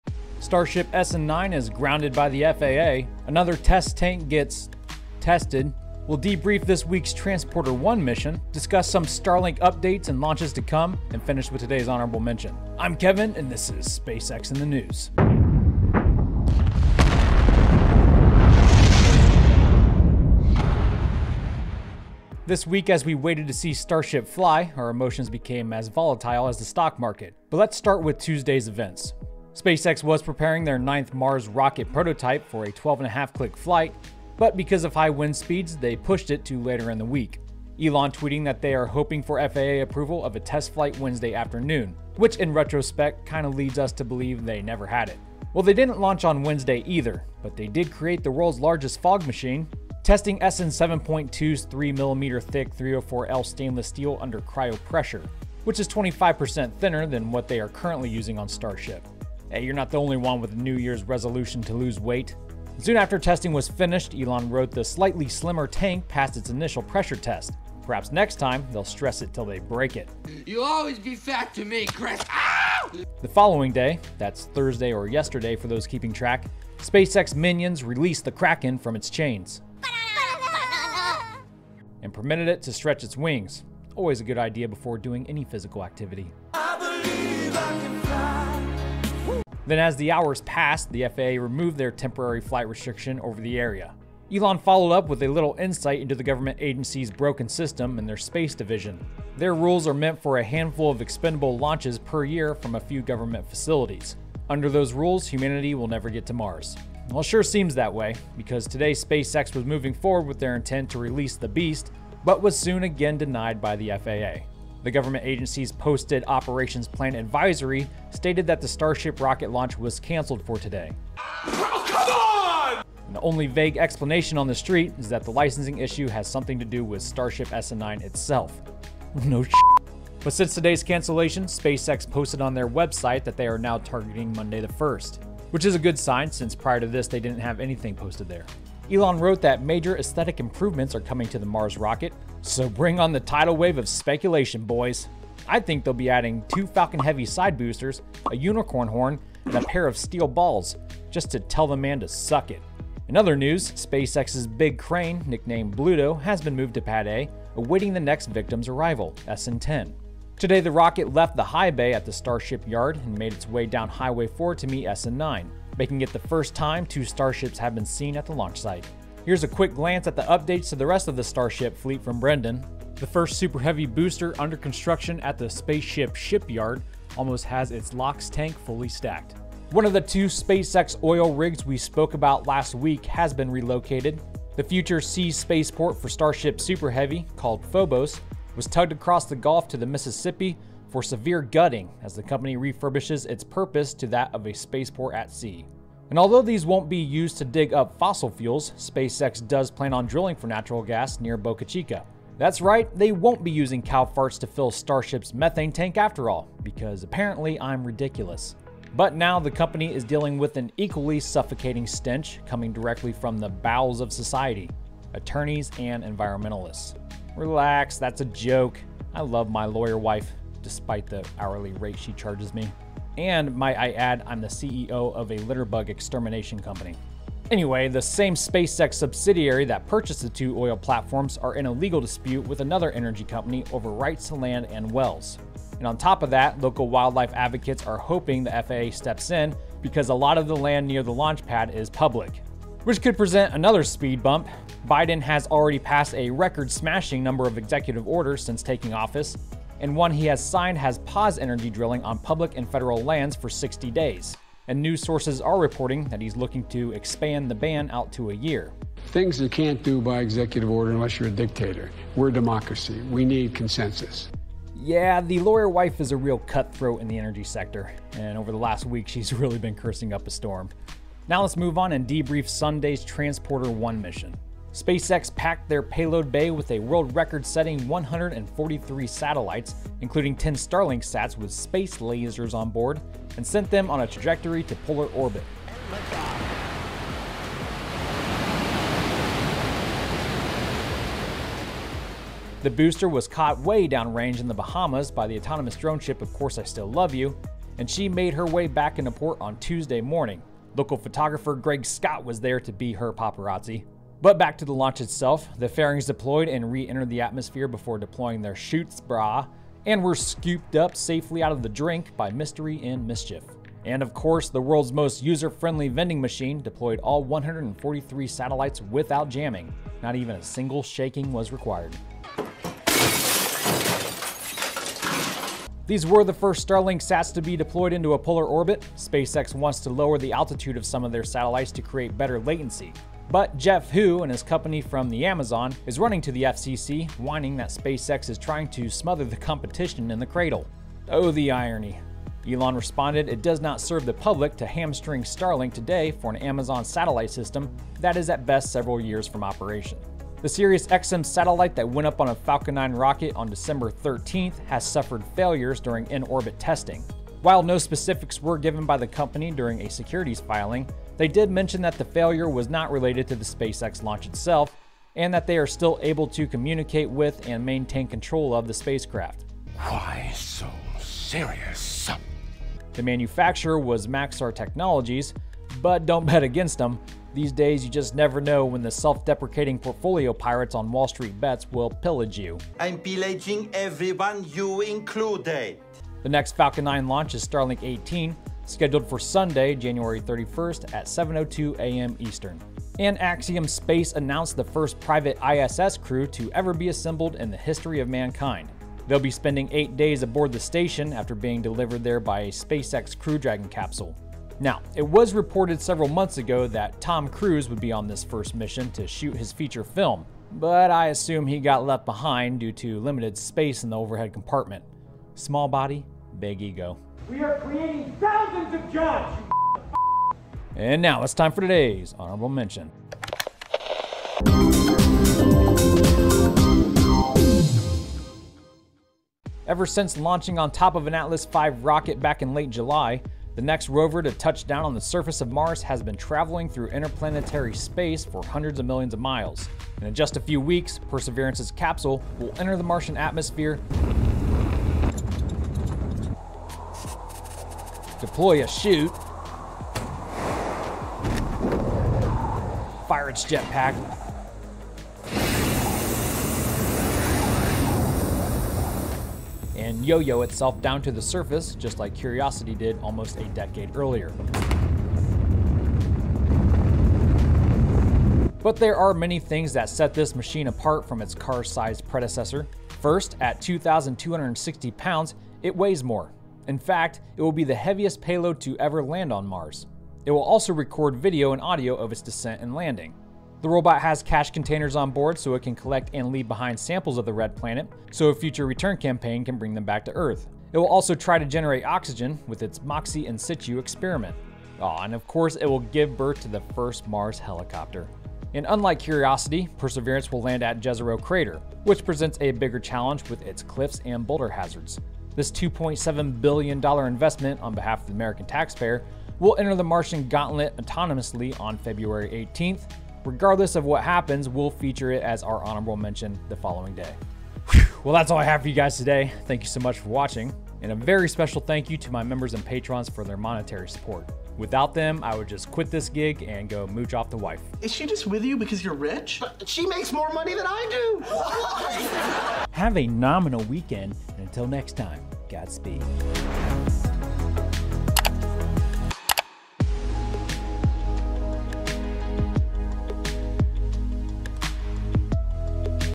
Starship Now Launching Monday, Second Starship Moved To Pad | SpaceX in the News
Episode 202. Today we catch up on the latest Starship updates. Debrief the recent Falcon 9 mission. Discuss Starlink news and what's to come. Then finish with today's Honorable Mention.

XCENTRIC PATCHES